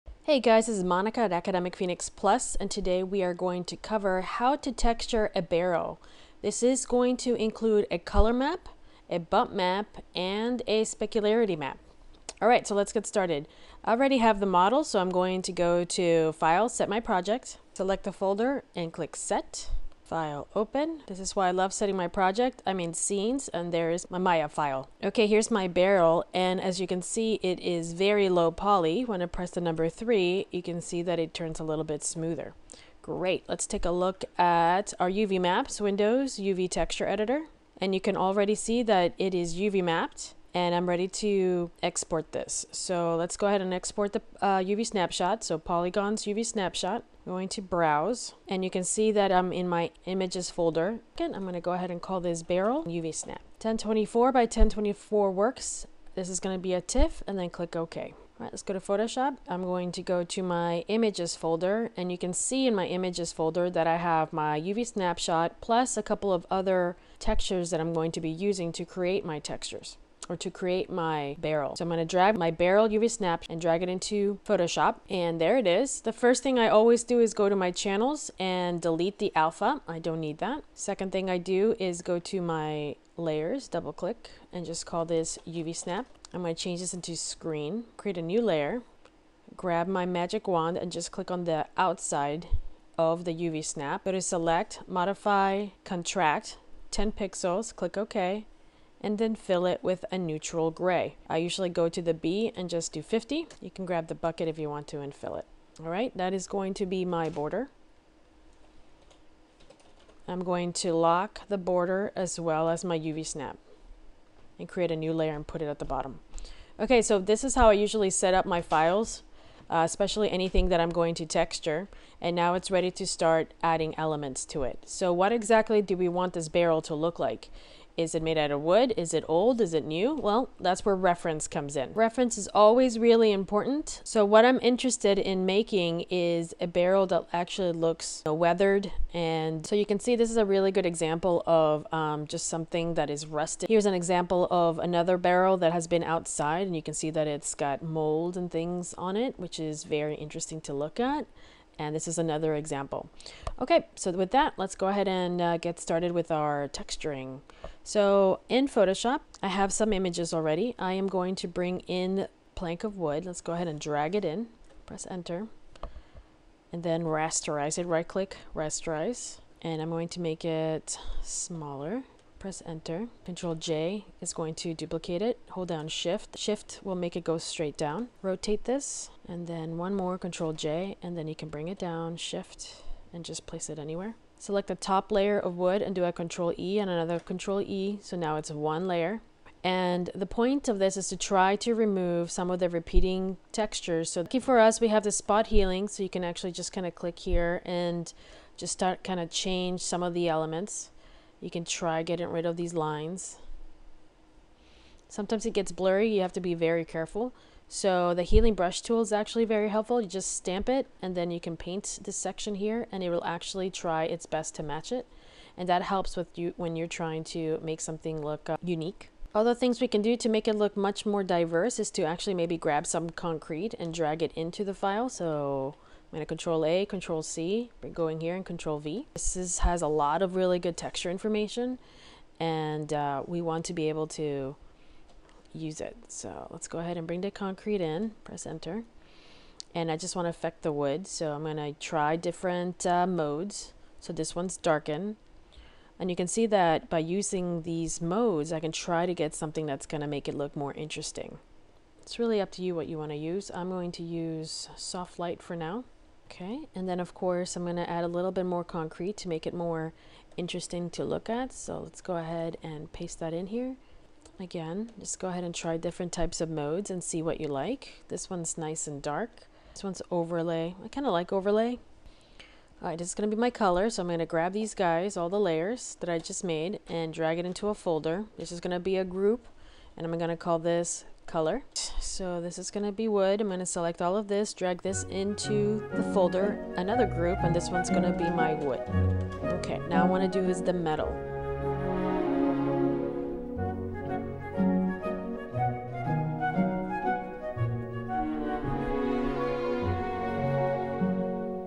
Texture a Barrel in Maya: Wood
This is a multi-part video tutorial on how to texture a barrel using Maya. We will create a color, bump and specularity map.

This video tutorial covers wood planks in a barrel.

Follow along! Download the FREE file here!

Programs used: Maya, Photoshop